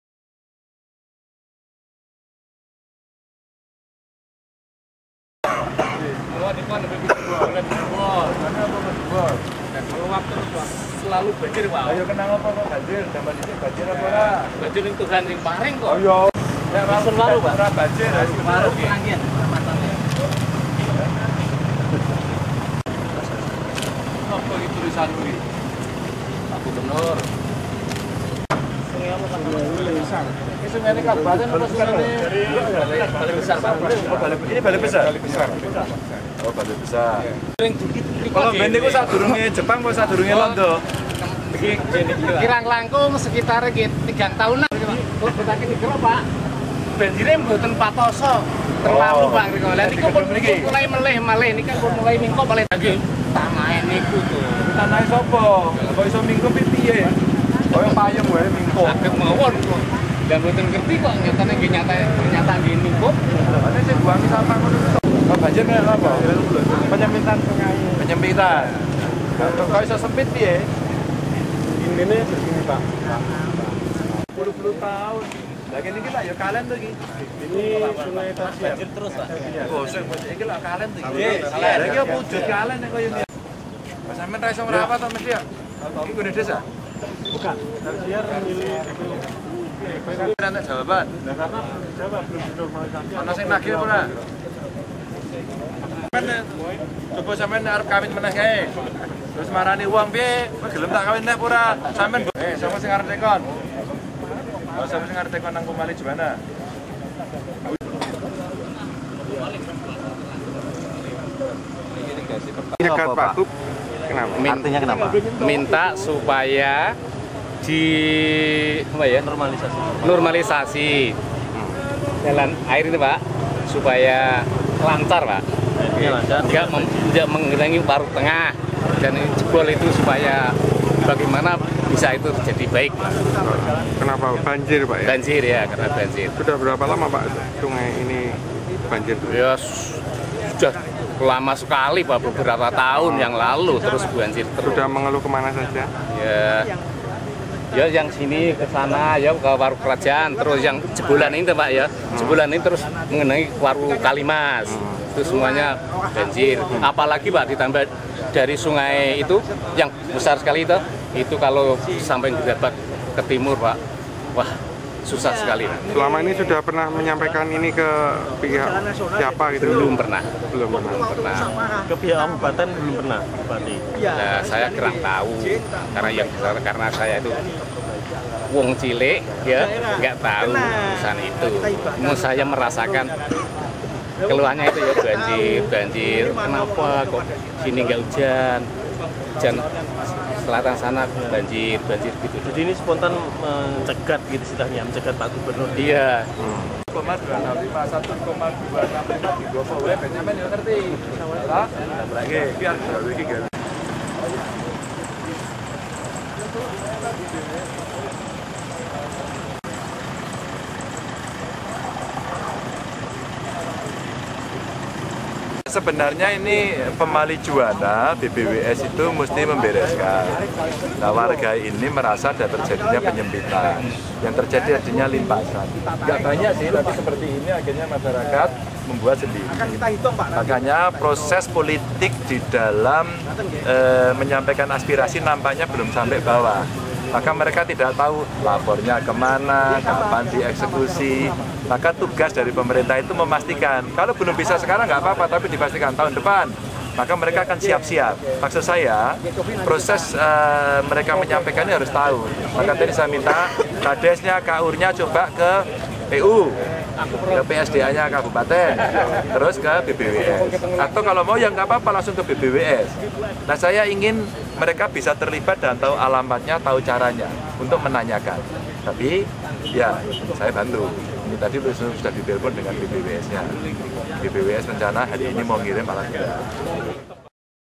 SUNGAI SERING BANJIR, WARGA HADANG ROMBONGAN GUBERNUR
Saat perjalanan lawatan ke Demak, Gubernur Jawa Tengah, Ganjar Pranowo dihadang lima orang warga di jalur Alternatif Waru Mranggen Demak, Rabu (18/1). Warga yang juga merupakan petani setempat menghadang rombongan Gubernur dengan membentangkan spanduk di pinggir jalan yang bertuliskan "Pak Gubernur Tolong Segera Dinormalisasi Sungai Ini Karena Sering Banjir", dengan cat semprot berwarna merah.

Melihat itu, Ganjar langsung turun dari mobil dan berdialog dengan warga tersebut. "Kenapa ini kok pasang spanduk segala, apa tulisannya ini?" tanya Ganjar kepada warga. Setelah membaca spanduk tuntutan warga, Gubernur langsung memeriksa aliran sungai yang kini kondisinya semakin menyempit. Bahkan di sisi seberangnya, sungai aliran dari sungai Kalimas ini nampak mampet, hanya genangan air saja.

Mendapati hal ini Ganjar kemudian meminta untuk segera menghubungi BBWS (Balai Besar Wilayah Sungai) Pemali Juwana. Dari keterangan yang didapat, besok pagi akan segera dikirim alat berat untuk melakukan pengerukan sungai. Ganjar menilai penyampaian aspirasi warga ini menurutnya keliru dengan melakukan penghadangan, seharusnya warga jika mendapati masalah segera lapor ke tingkat bawah agar cepat diteruskan ke dinas terkait. Namun dengan ini sangat mengapresiasi bahwa setidaknya warga masih peduli dengan lingkungannya.